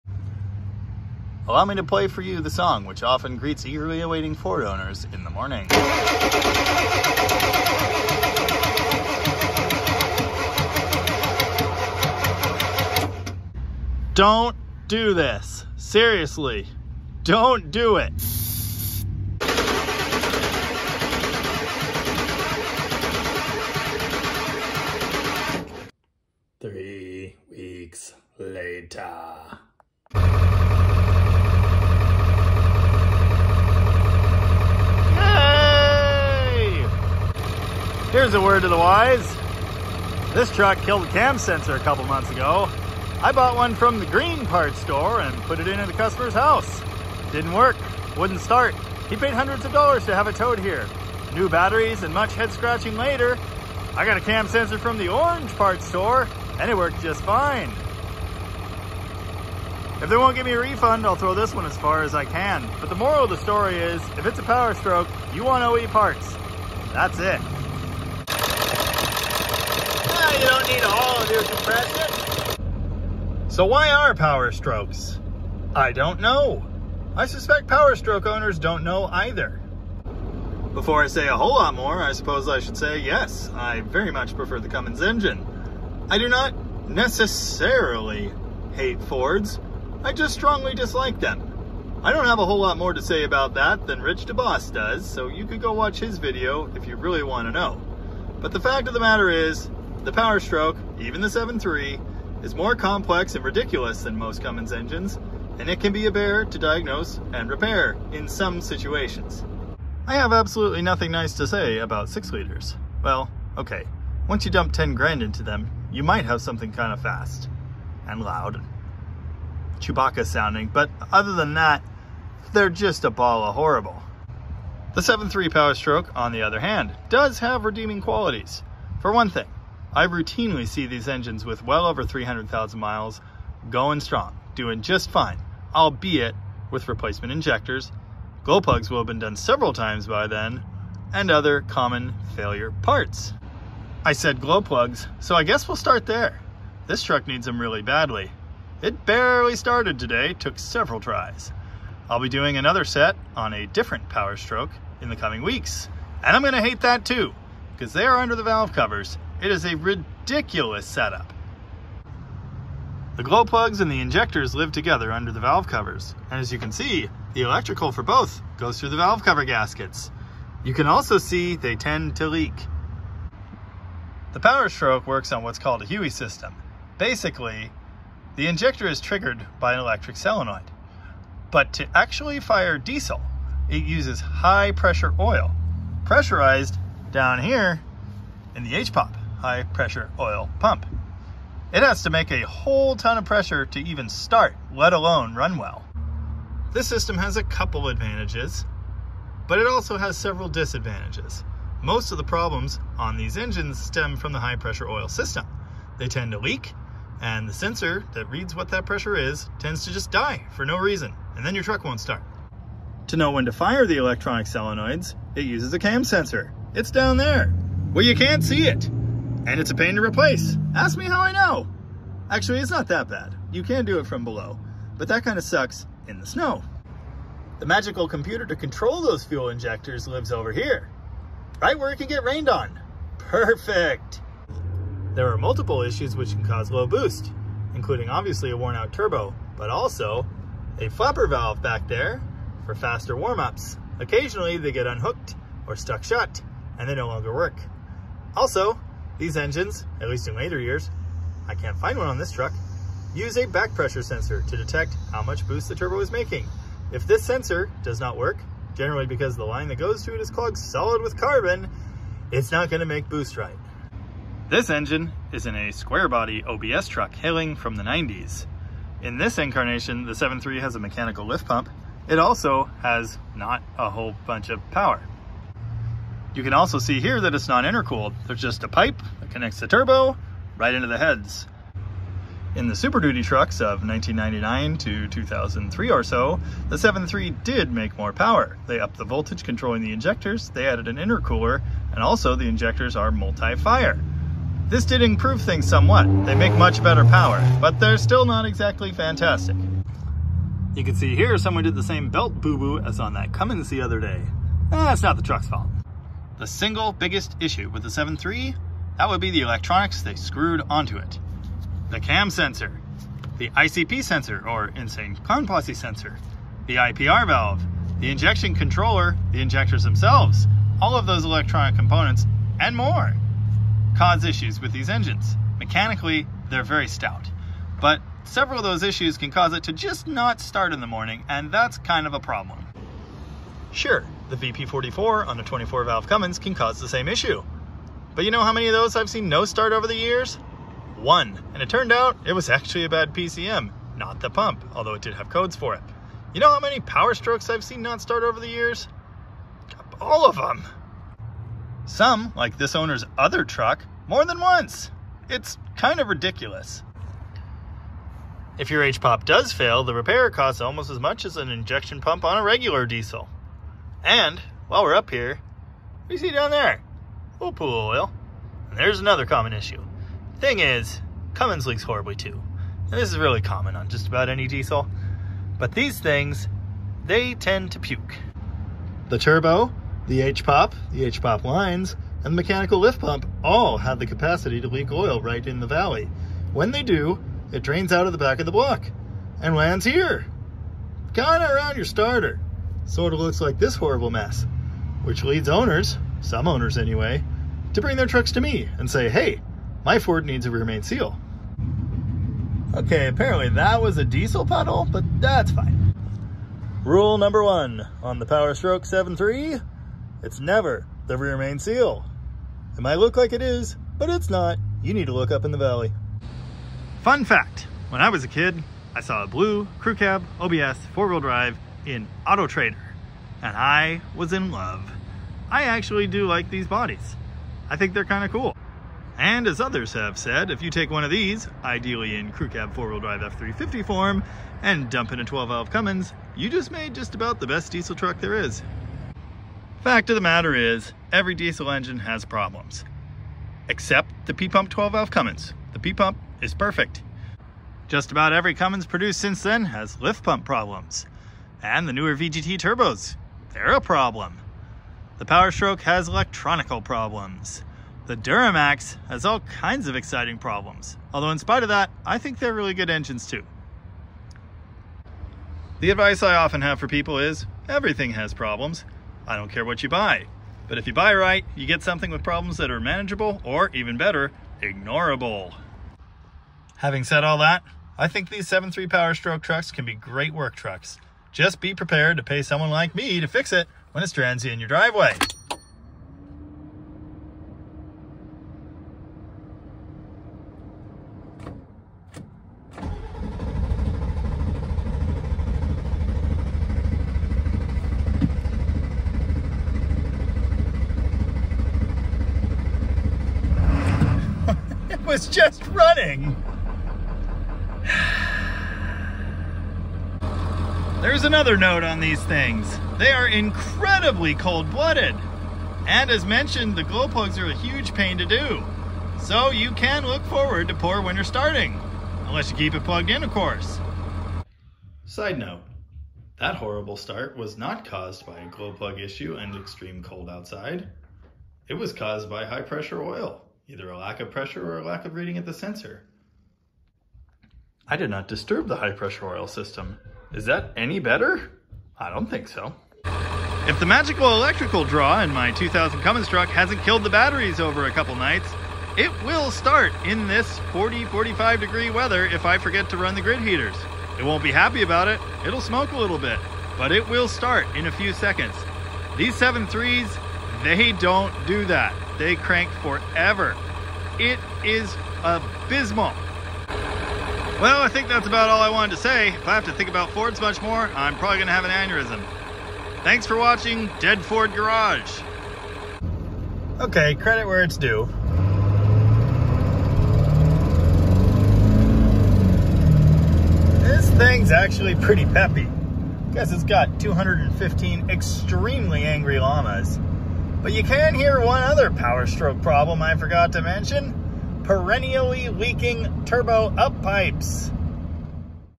Just 7.3 Powerstroke Problems - A Mechanic’s Perspective - Dead Dodge Garage
I detail some of the annoying problems I’ve had to fix on 7.3 Ford / Navistar Powerstroke Diesel engines over the years. As I view them from the perspective of a mechanic, I basically only see them when they’re broken - but break they do.

Oh, and I have been reminded that the exhaust back pressure sensor and pipe are on the front of the 7.3. I was looking for it in the 6.0 position. If you came here expecting perfection…